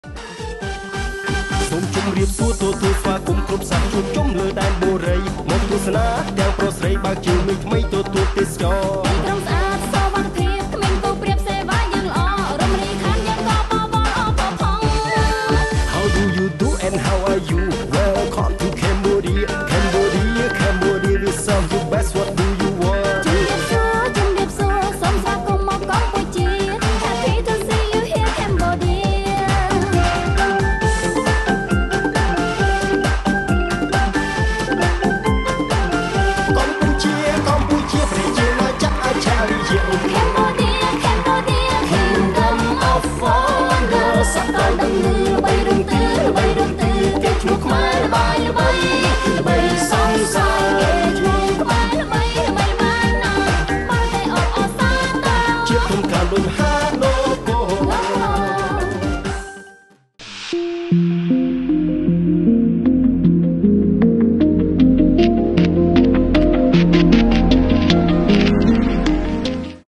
923-សូមរីករាយទស្សនា វីដេអូរ រថយន្ត ៤៥កៅអី ទំនើបៗរបស់ក្រុមហ៊ុន ប៊ែលធី ទេសចរណ៍
សូមរីករាយទស្សនា វីដេអូរ រថយន្ត ៤៥កៅអី ទំនើបៗរបស់ក្រុមហ៊ុន ប៊ែលធី ទេសចរណ៍
Please enjoy watching the video of modern 45- seats buses of BELTEI Tours & Travel